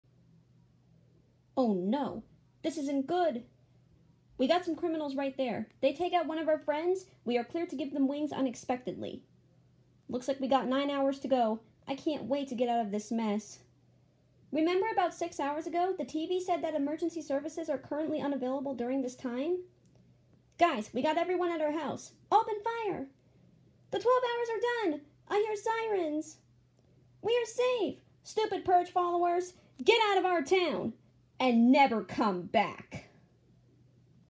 My lines for The Purge for ItsYaBoiApollo
Requested by ​@ItsYaBoi-Apollo
He gave me some lines for a new animation he is making, and here is me saying them! I added a little extra line for my last line. I hope my lovely friend Apollo doesn’t mind.